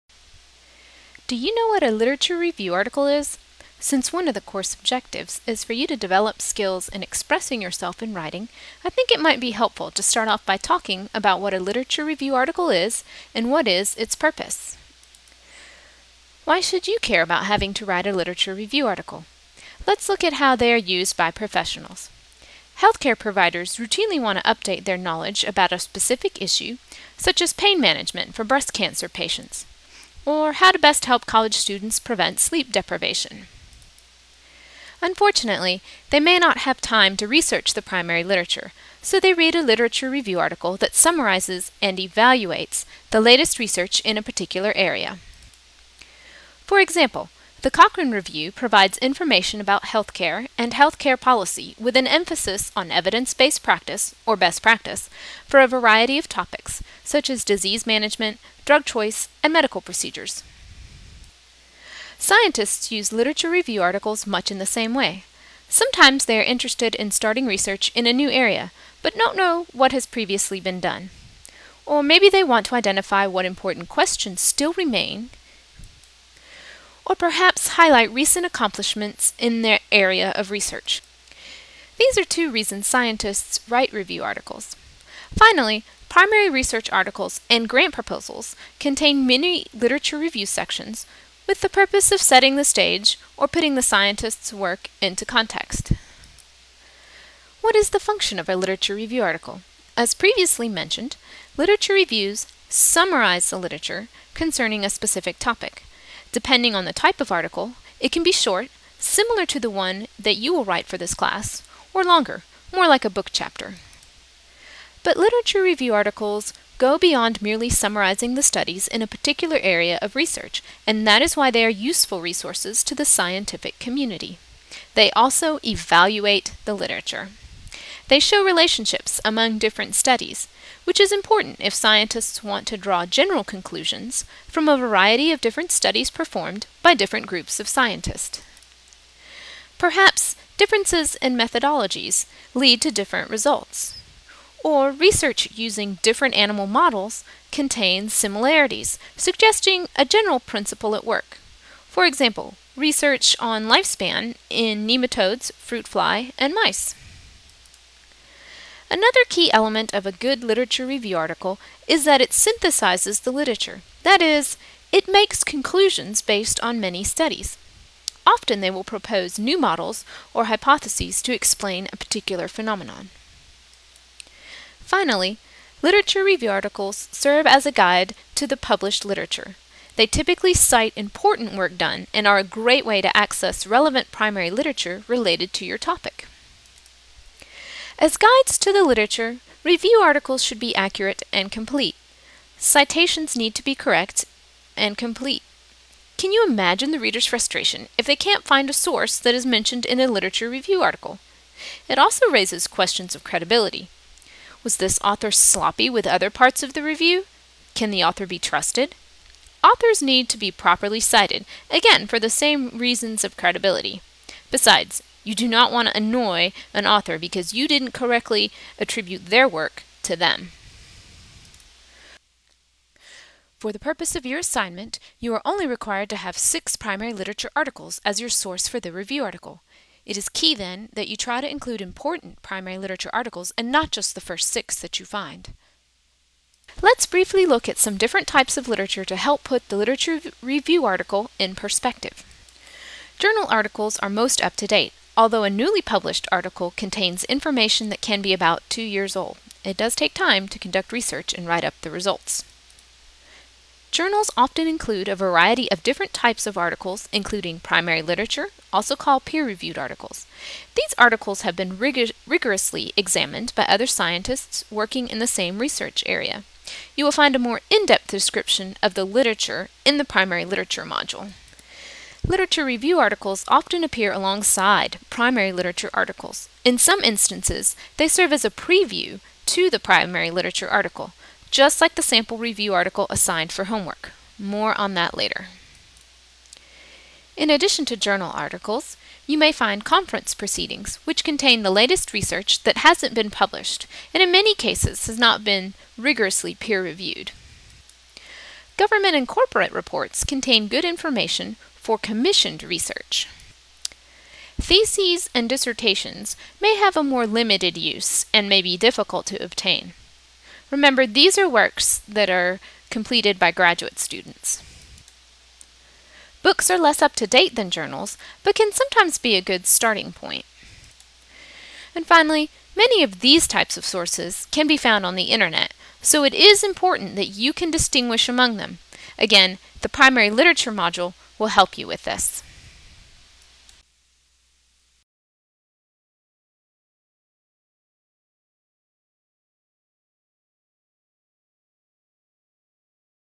What is a Literature Review Article?
A brief introduction to a scientific literature review article